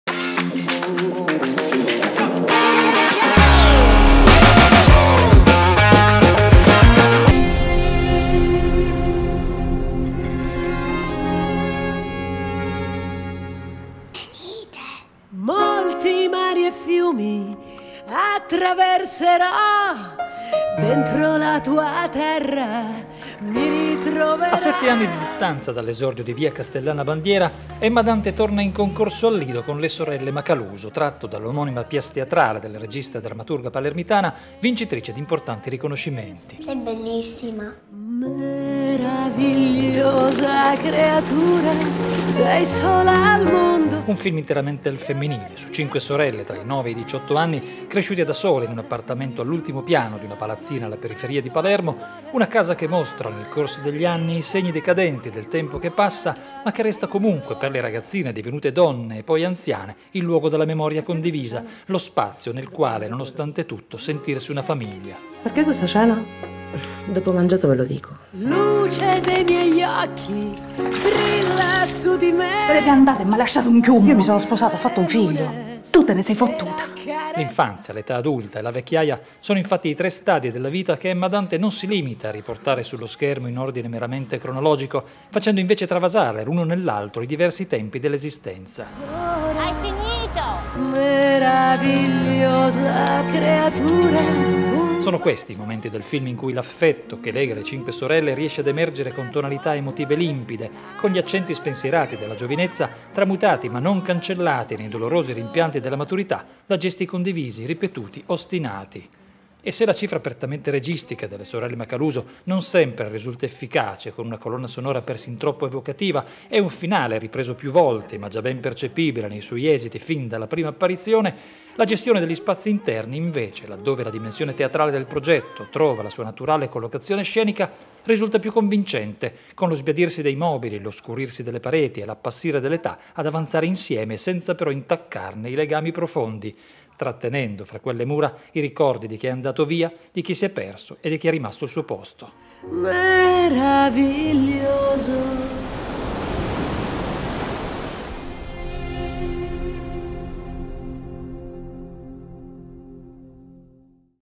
LE SORELLE MACALUSO di Emma Dante
Paolo Perrone di Filmcronache dal Lido recensisce LE SORELLE MACALUSO di Emma Dante, in concorso a Venezia77.

Video: Paolo Righini